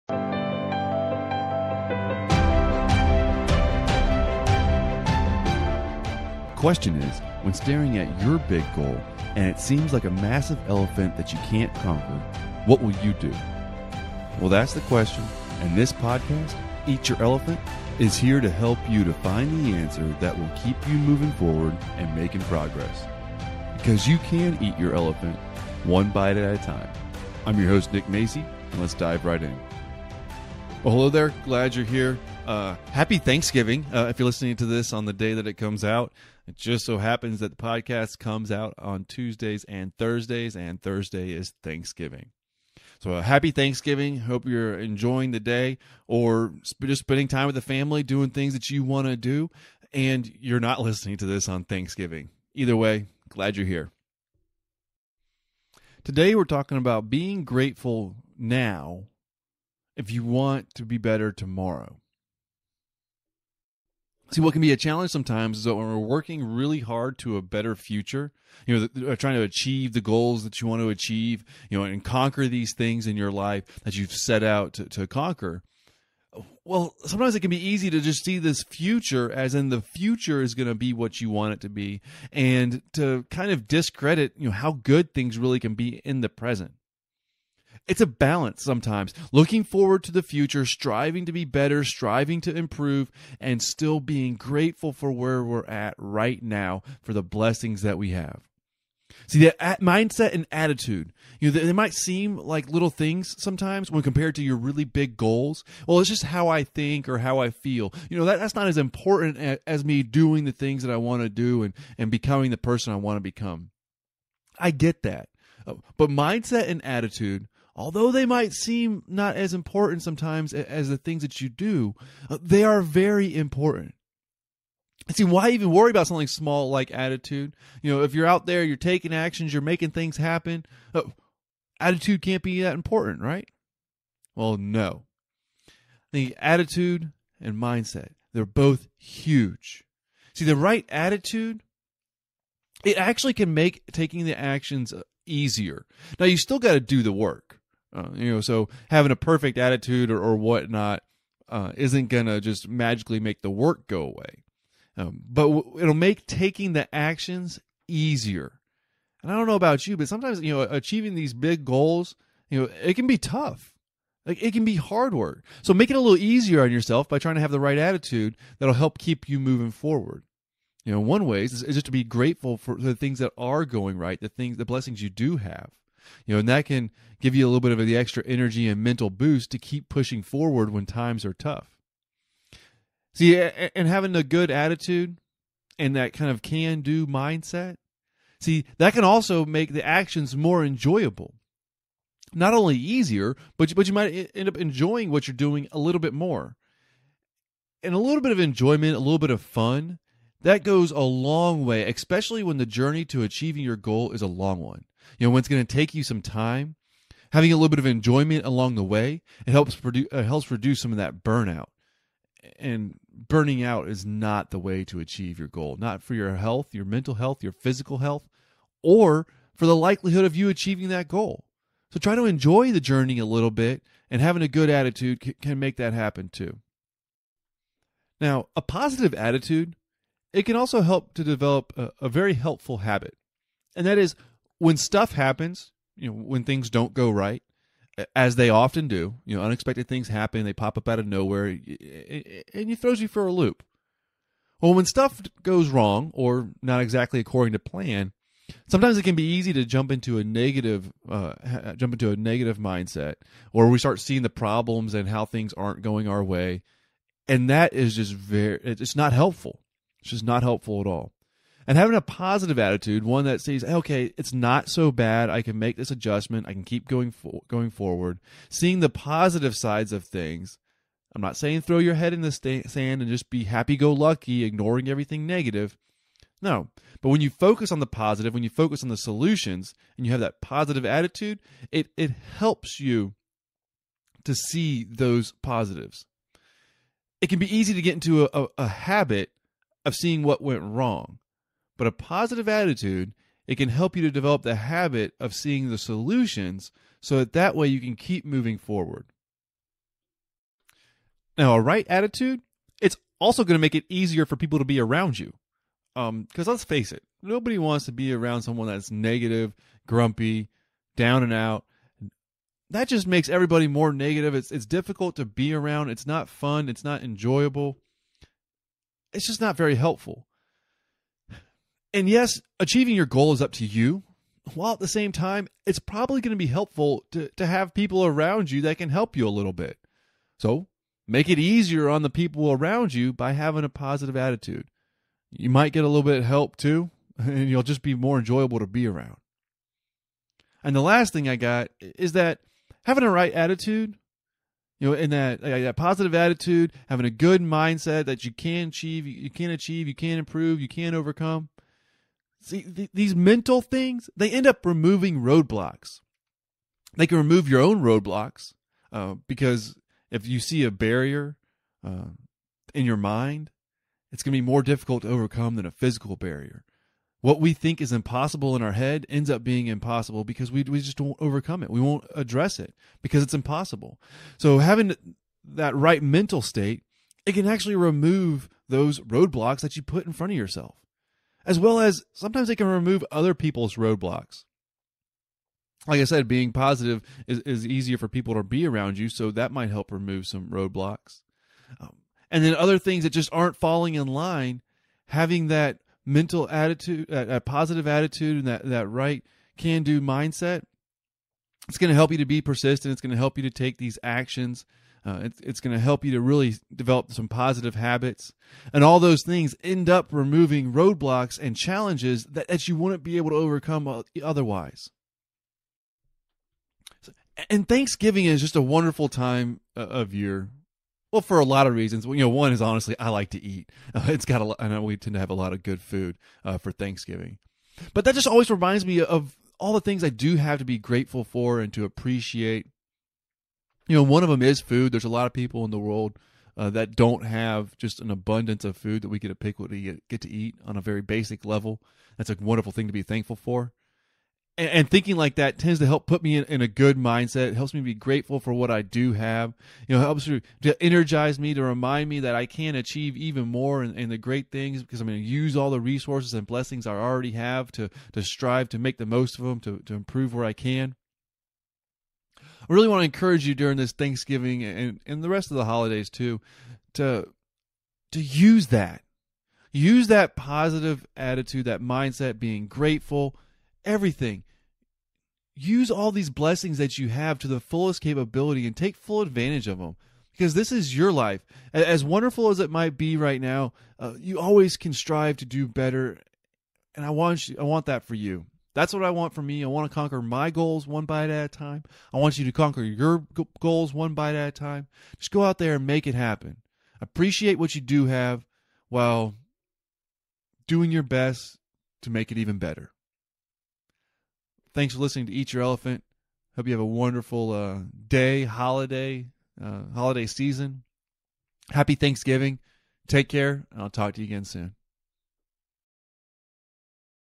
Be Grateful For Now If You Want A Better Tomorrow
Mindset & Attitude - They may seem like little things when compared to your big goals.

Why even worry about something like attitude? Aren’t you out there taking actions and making things happen? Your attitude can’t be that important, right?

NO!

The Right Attitude Helps
• Makes action easier
• Makes action more enjoyable
• Develops a helpful habits
• Easier to be around for people who can help you
• Removes roadblocks